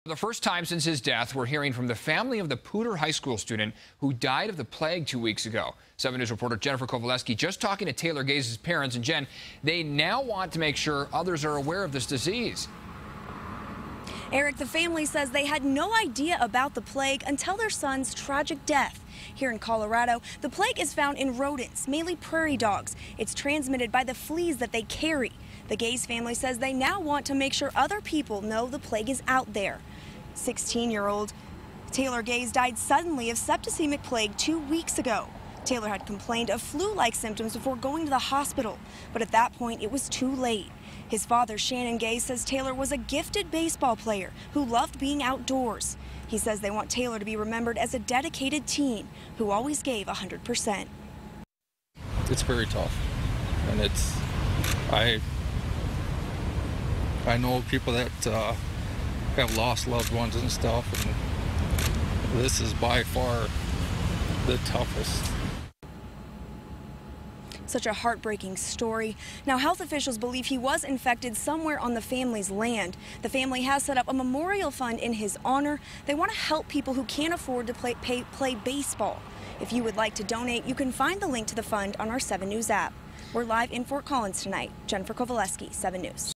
Colorado plague victim's family speaks
The family of a Fort Collins teen spoke publicly Tuesday for the first time since his death.